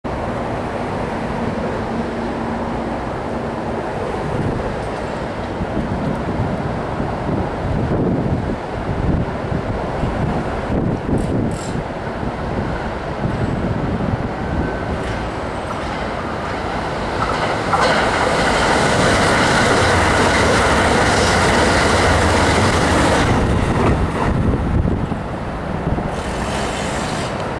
JR西日本 キハ189系6連特急はまかぜ1号 ＠塚本 2011/5/14
ご覧いただきましてありがとうございます。昨年からキハ181系に代わって、はまかぜ号として活躍しているキハ189系、軽やかに塚本駅を通過していきました。